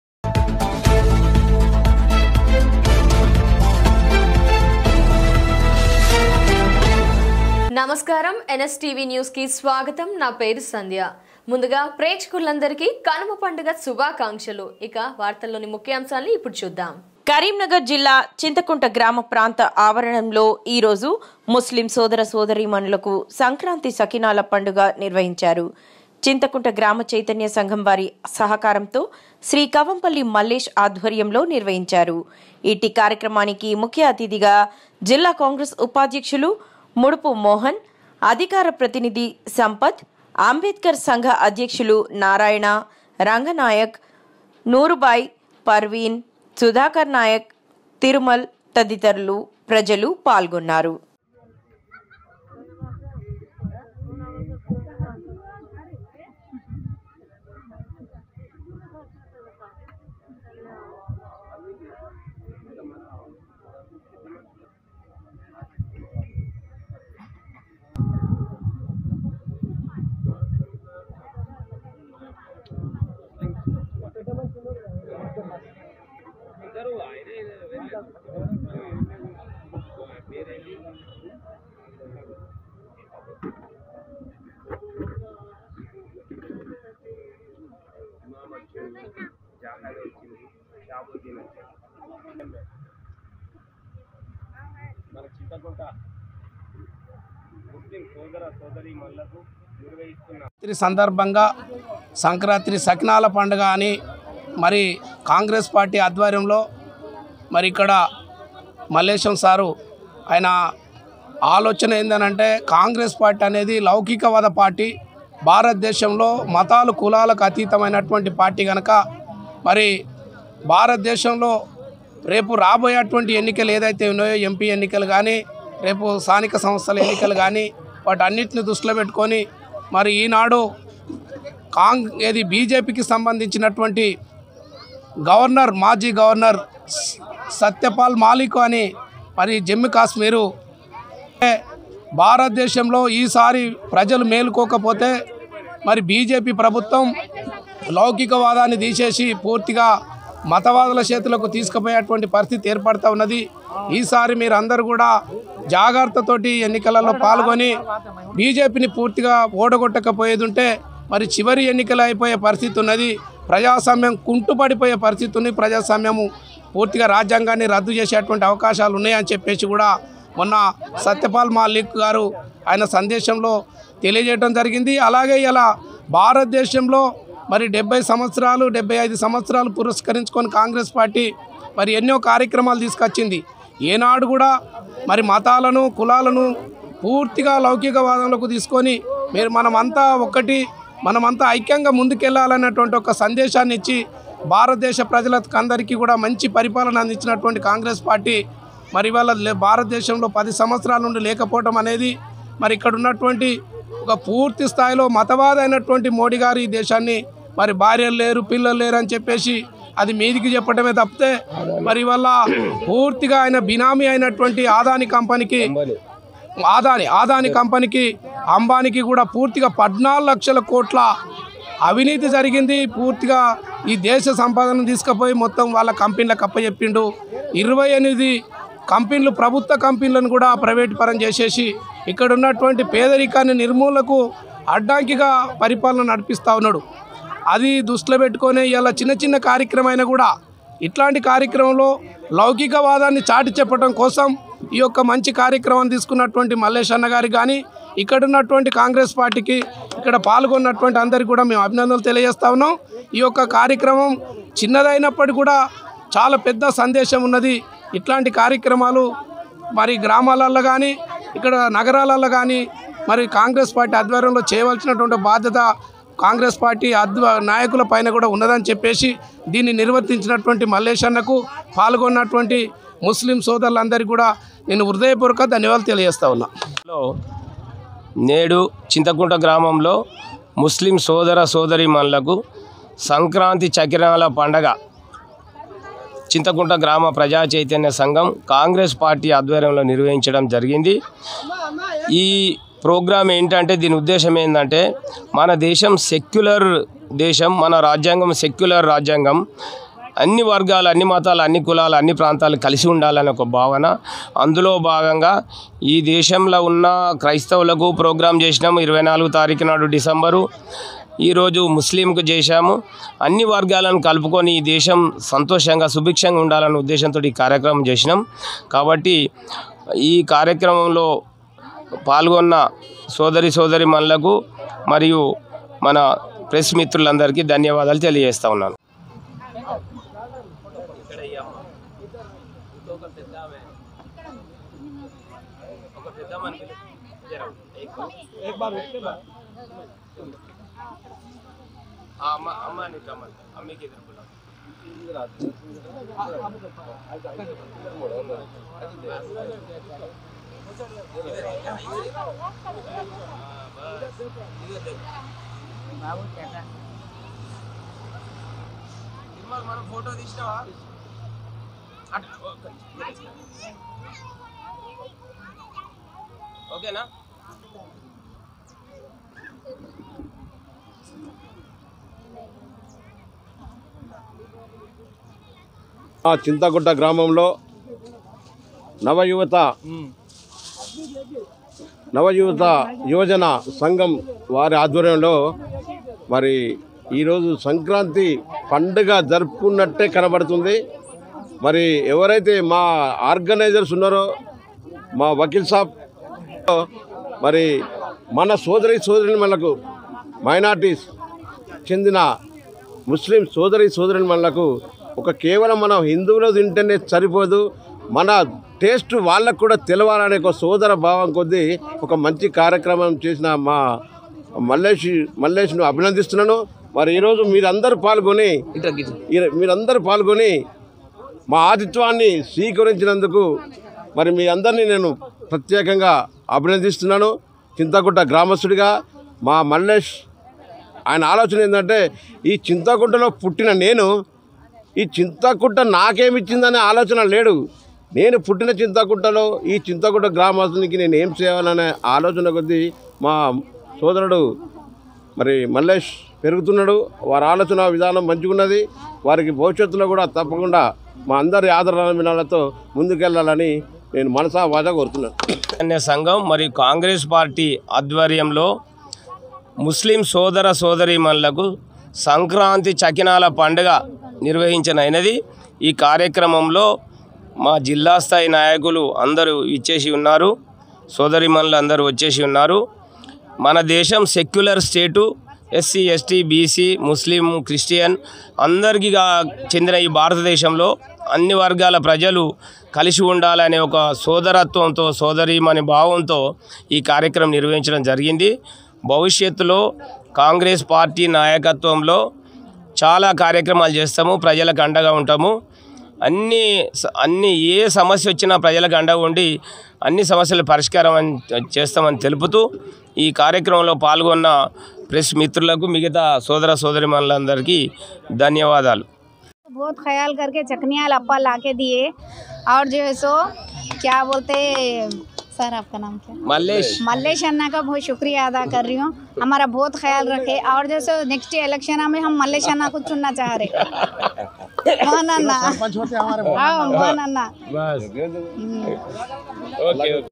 కరీంనగర్ జిల్లా చింతకుంట ఆవరణంలో ఈరోజు ముస్లిం సోదర సోదరీమణులకు సంక్రాంతి చకినాల పండుగ నిర్వహించారు
కరీంనగర్ జిల్లా చింతకుంట ఆవరణంలో ఈరోజు ముస్లిం సోదర సోదరీమణులకు సంక్రాంతి చకినాల పండుగ నిర్వహించారు ||karimnagar sankranthicelebrations sankranthi sankranthifestival @NSTVlive